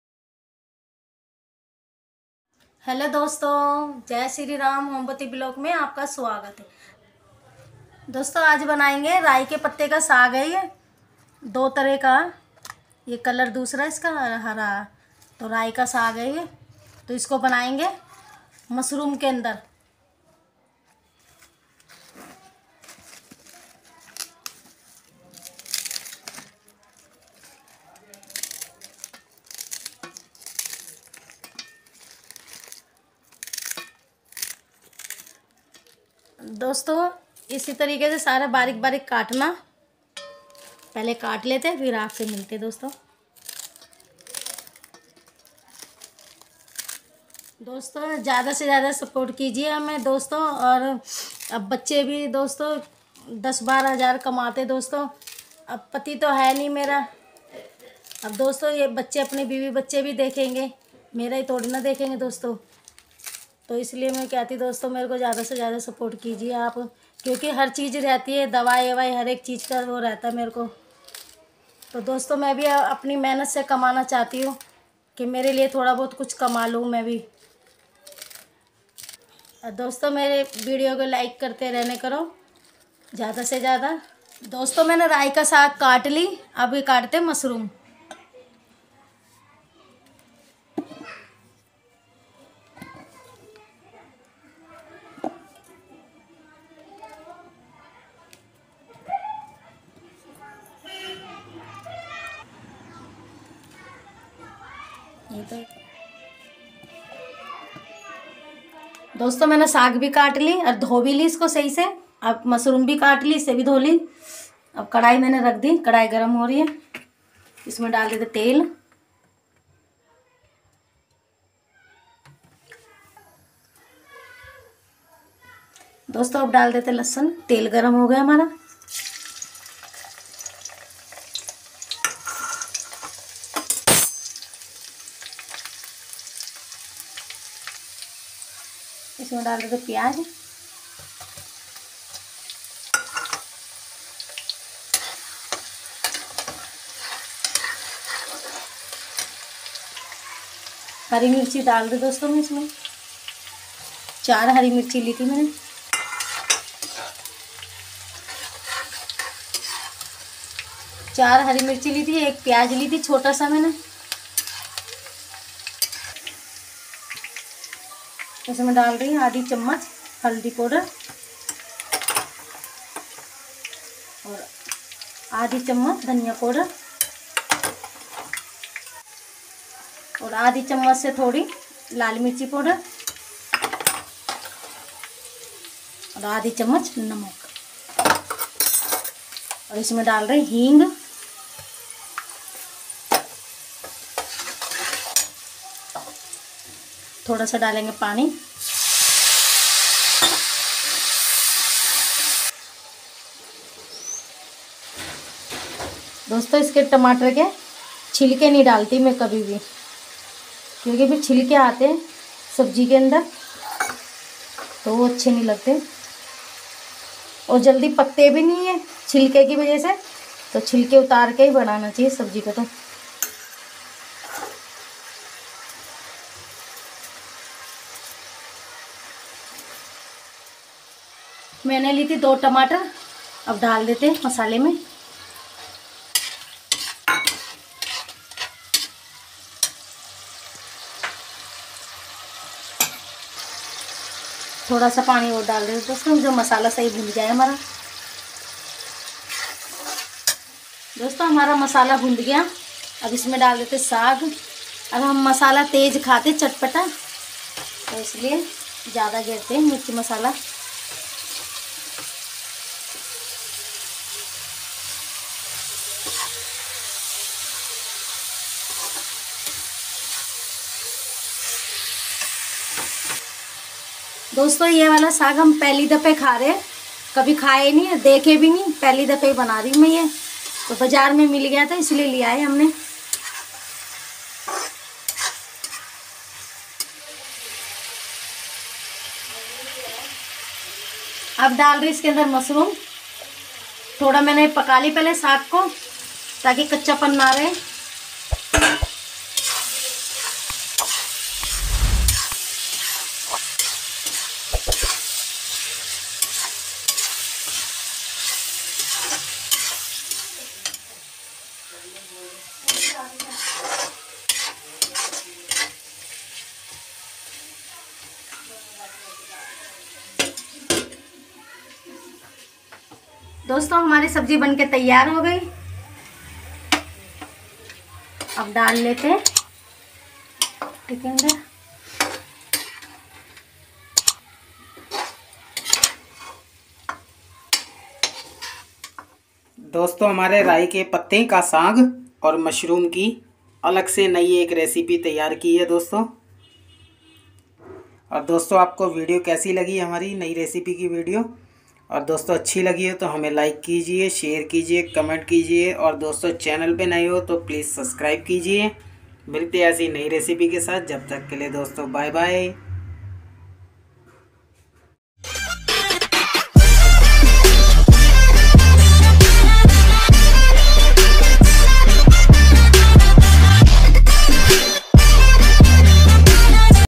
YouTube Channel Pe Ek New Recipe || राई के पत्ते का साग, मशरुम के साथ || Aap Bhi Try Kare || Dosto 👍
Raai Ke Patte Ka Saag. Mashroom Ke Sath

Verry Tasty 😋 Recipe Dosto